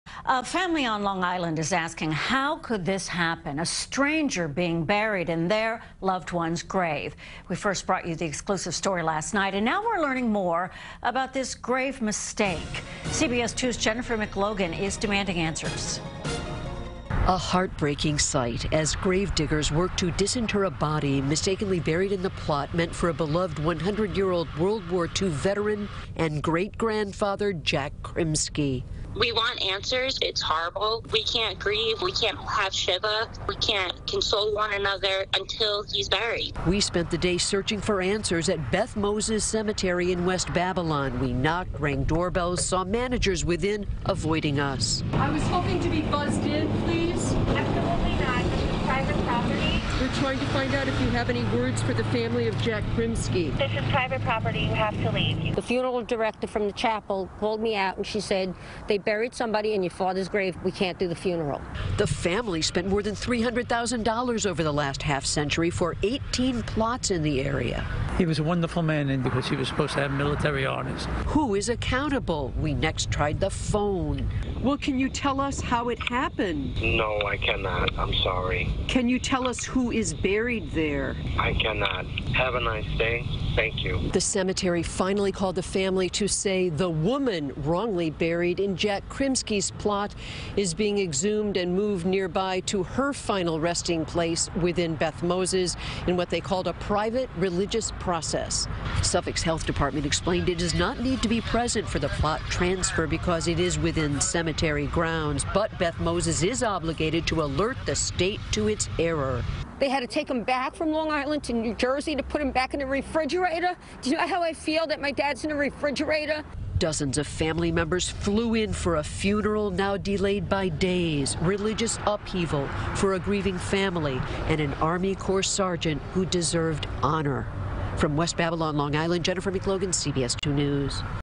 Demanding Answers: Family Wants Accountability After Stranger Is Buried In Loved One's Grave At Long
A family on Long Island is asking how a stranger could be buried in their loved one's grave; CBS2's Jennifer McLogan is demanding answers.